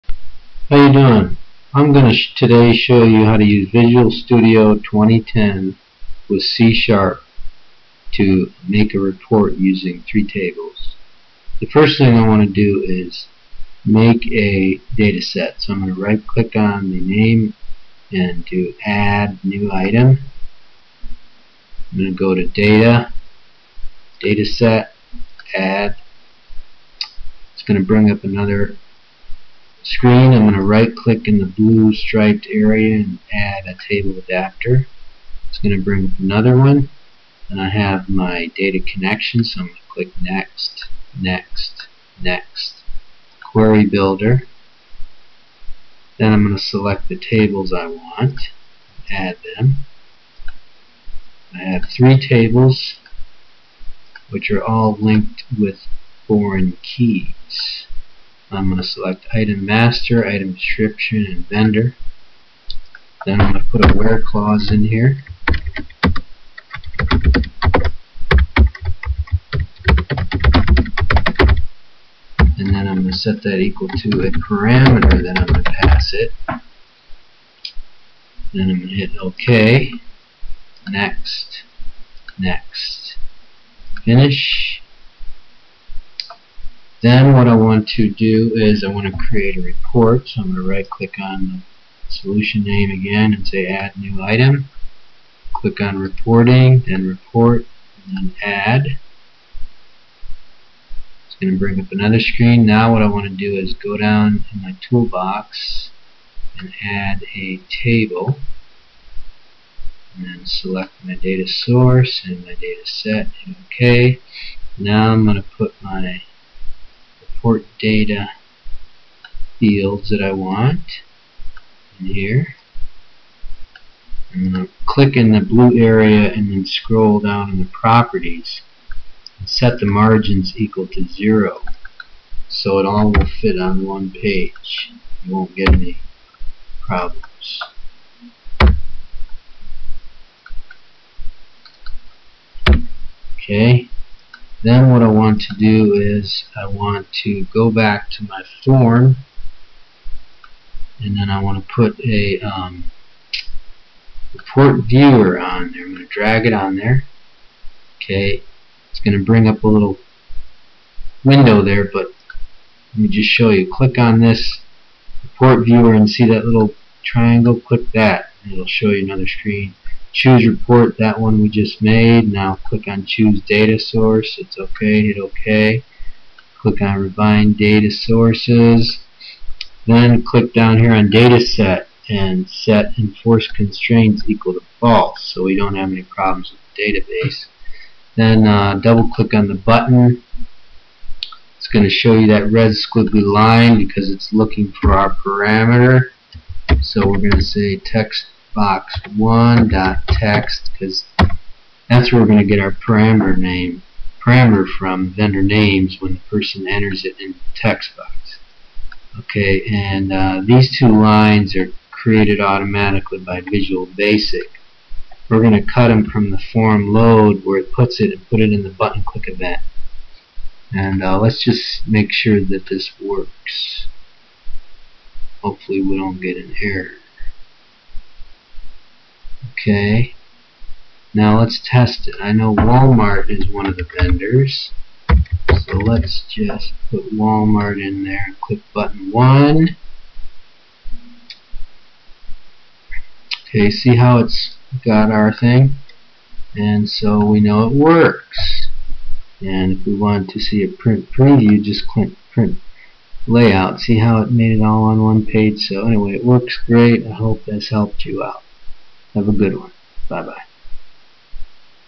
How to Create a Query-based Report in Microsoft Visual Studio 2010 using C#
Make a report using Visual Studio 2010 and C# based on a dataset which queries three tables. Based on a string entered by user, database is queried and a report is produced. Learn how to stop the report from printing multiple blank pages and avoid referential integrity constraint errors.